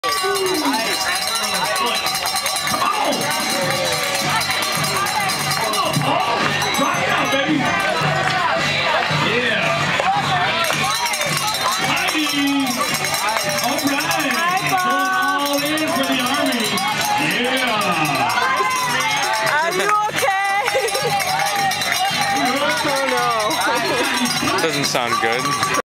IMAZ2008RunSuffer.avi
Ironman Arizona Run - November 23, 2008 - My husband Paul Zieske, Age 48 is still suffering, says "Pray for me!"